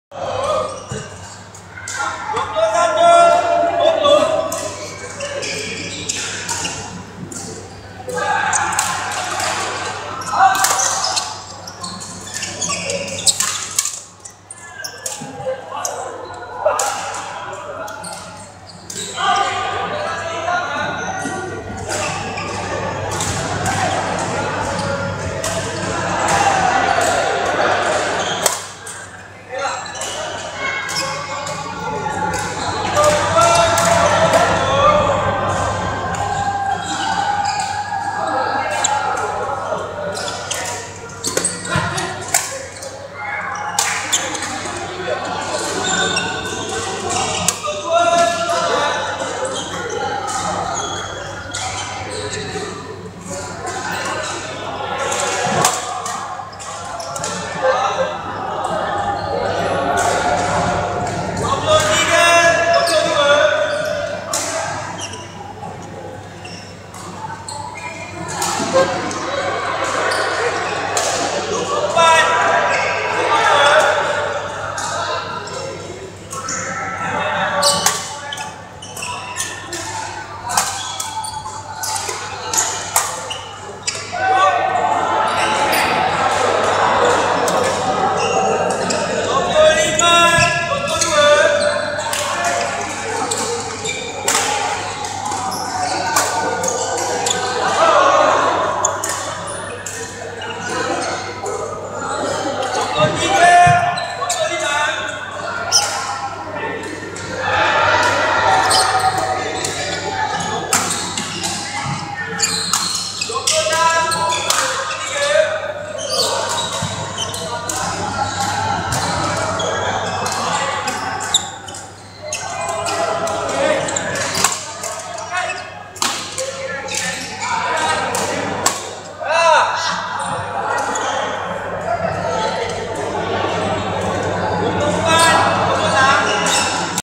Zafree/Nasrul vs Razali/Opie (Peringkat Liga)
Pertandingan Liga Badminton Tertutup C16 - 12 Mei 2017